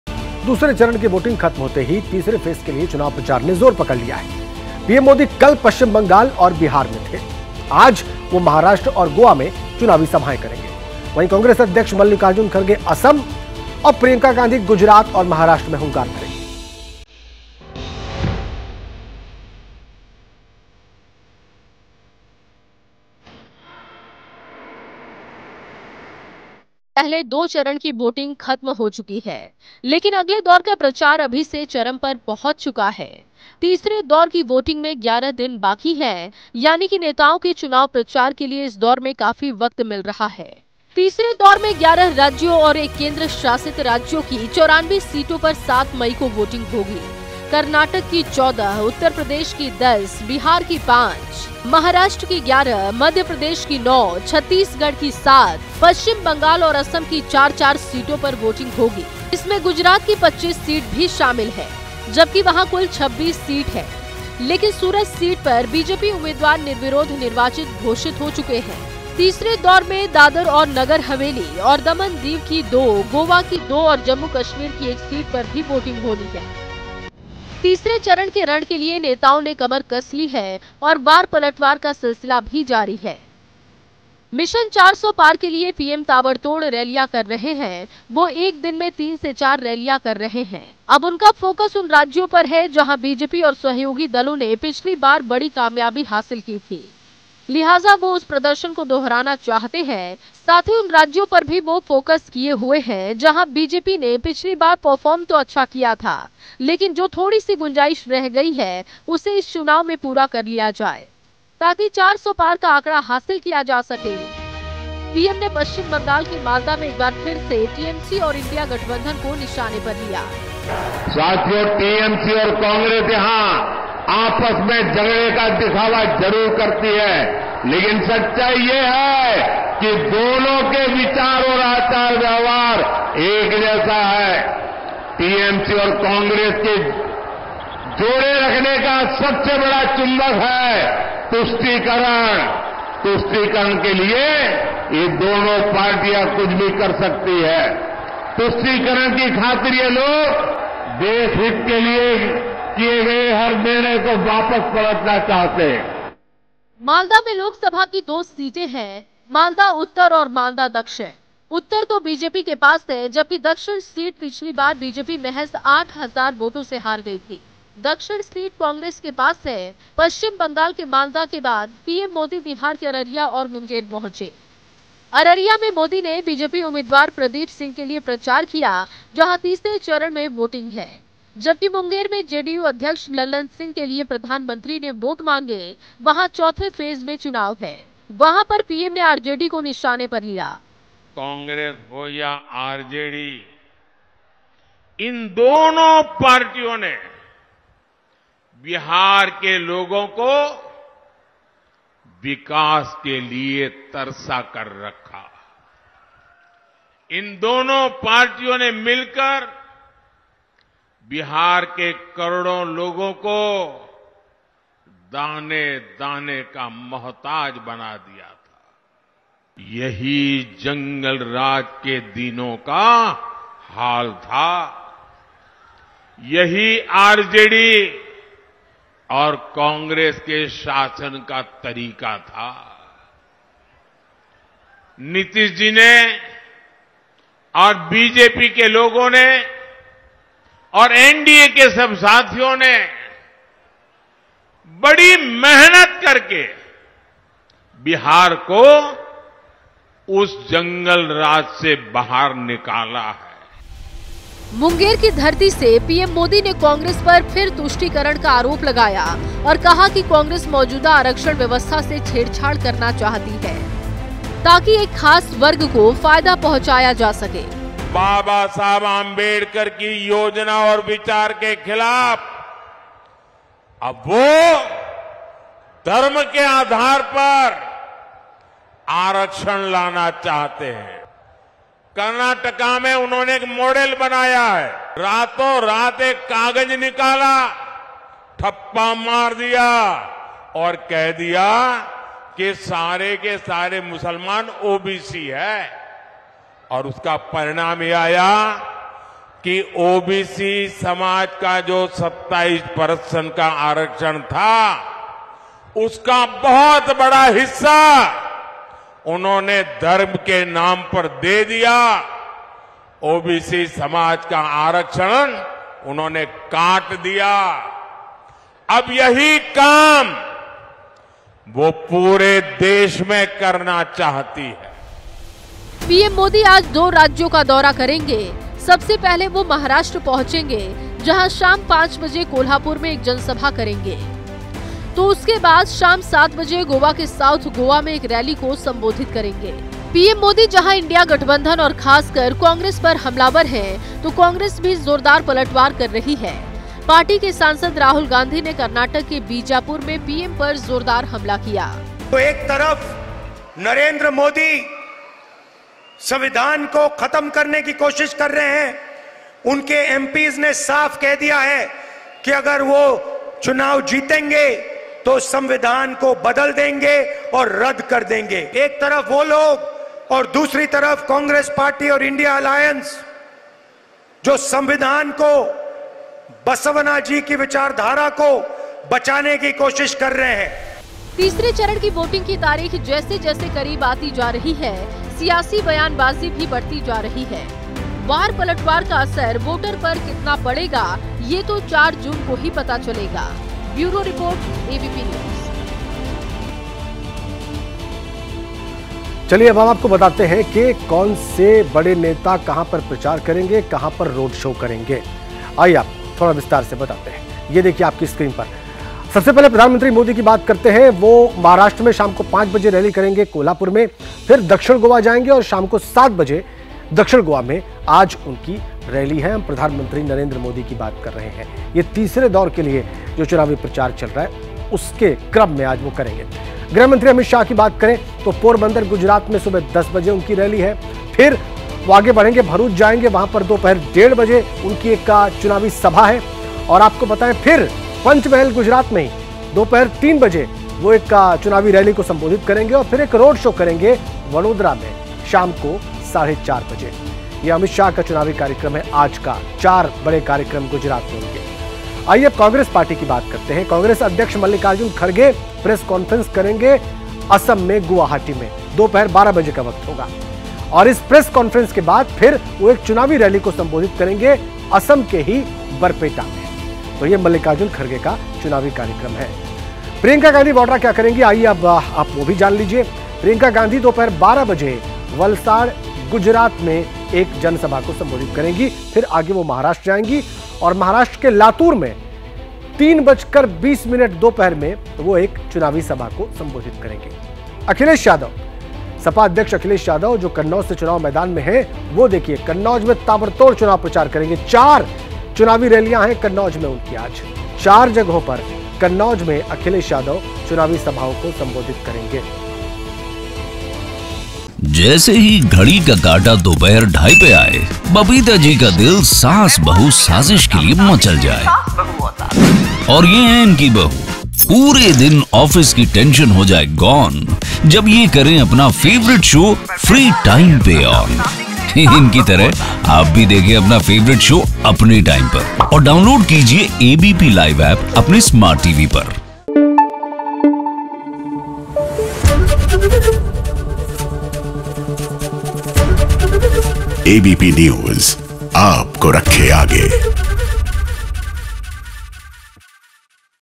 Elections 2024: बयानों की बौछार के बीच तीसरे दौर के चुनाव के लिए प्रचार ने पकड़ी रफ्तार | ABP News
दूसरे चरण की वोटिंग खत्म होते ही...तीसरे फेज के लिए चुनाव प्रचार ने जोर पकड़ लिया है...पीएम मोदी कल पश्चिम बंगाल और बिहार में थे...तो आज वो महाराष्ट्र और गोवा में चुनावी सभा करेंगे...वहीं कांग्रेस अध्यक्ष मल्लिकार्जुन खरगे असम...तो प्रियंका गांधी गुजरात और महाराष्ट्र में हुंकार भरेंगी...